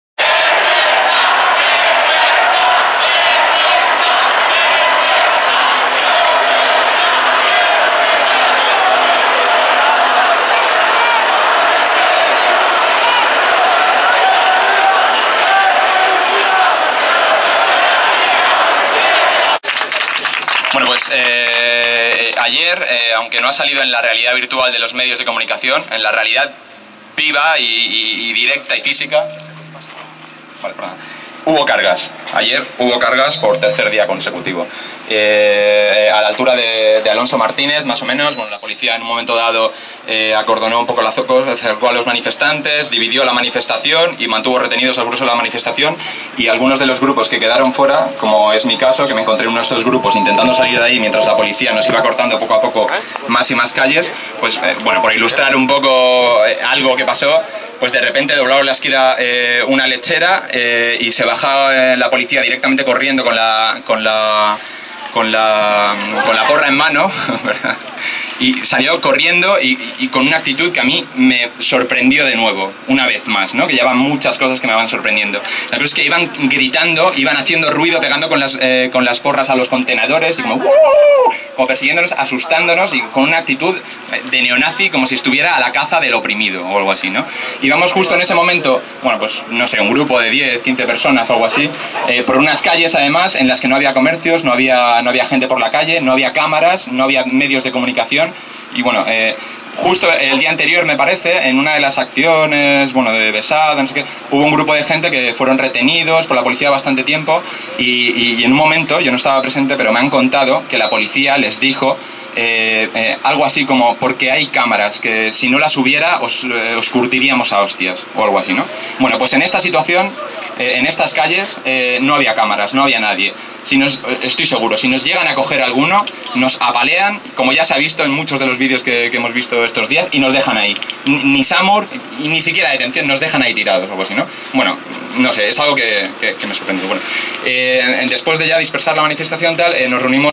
Testimonios de la violencia de estado
Testimonios de víctimas de la violencia de estado, de la brutalidad policial, y del estado de sitio policial en Madrid durante la visita de ratzinger y sus fieles     Madrid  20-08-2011